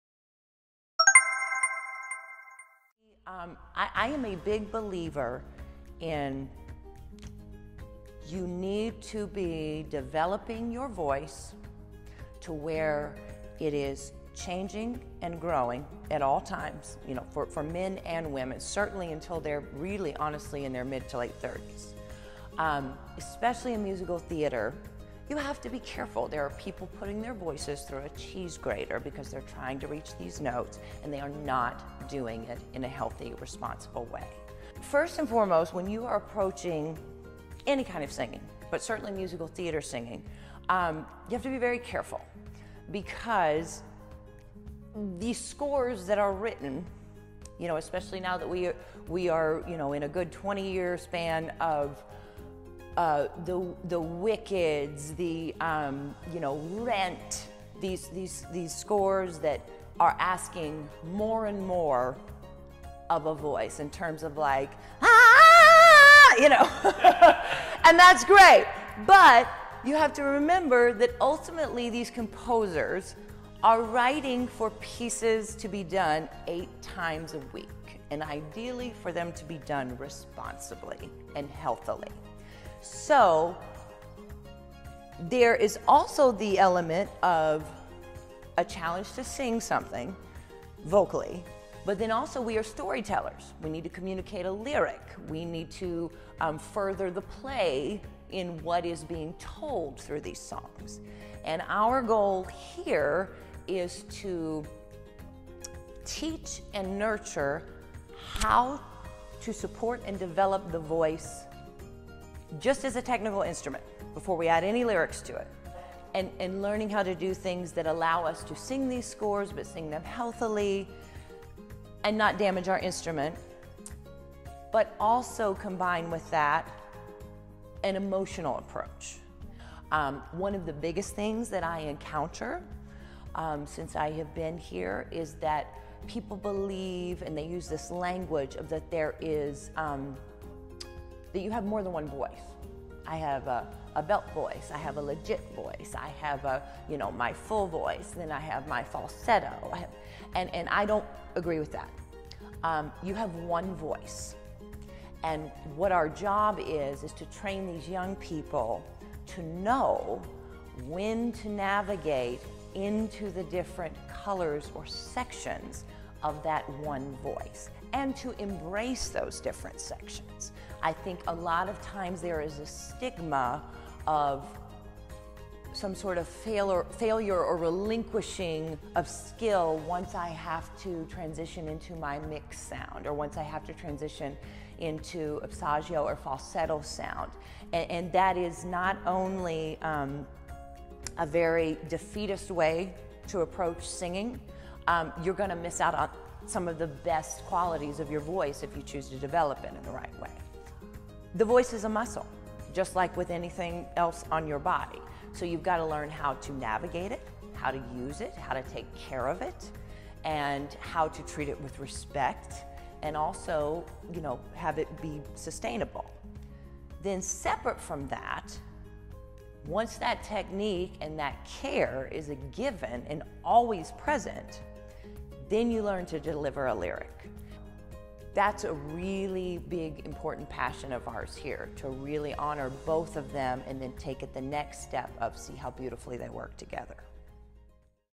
Sara Gettelfinger - Singing and Vocal Training at Performing Arts Louisville
Director of the Barbara F Cullen Musical Theatre Training Program, Sara Gettelfinger, talks about her philosophy for vocal training in order to develop a sustainable, healthy singing instrument.